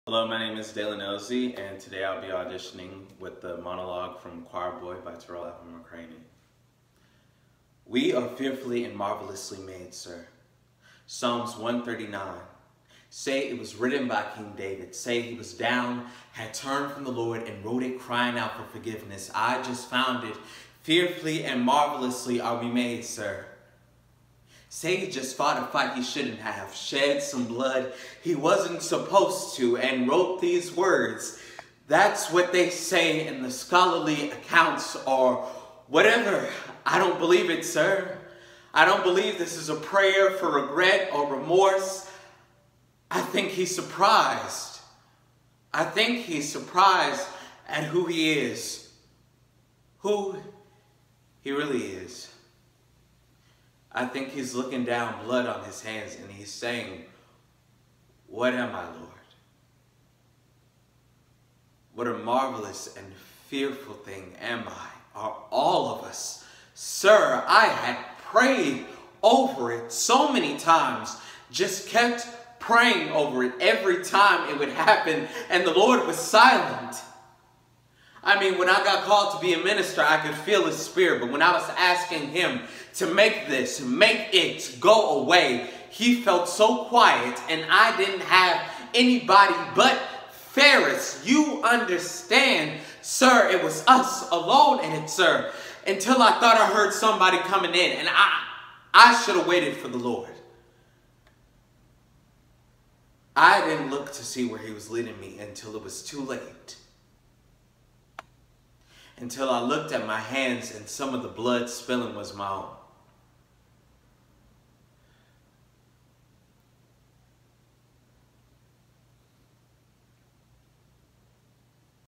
Choir Boy Audition Monologue
I will be auditioning with material from the play Choir Boy by Tarell Alvin McCraney as the character David.